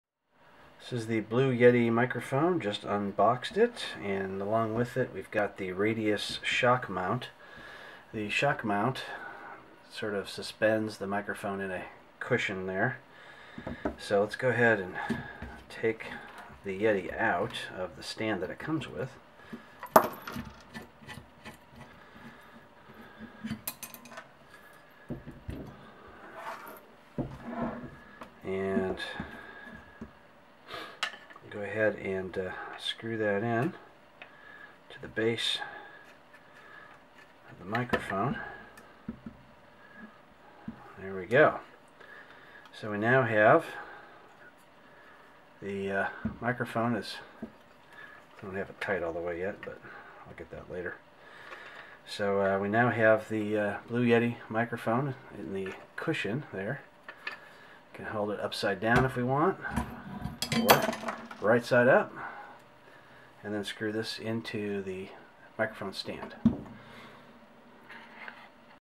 Mounting the Yeti Blue microphone into the Radius Shockmount
Author, teacher and talk show host Robert McMillen shows you how to mount the Yeti Blue microphone into the Radius Shockmount.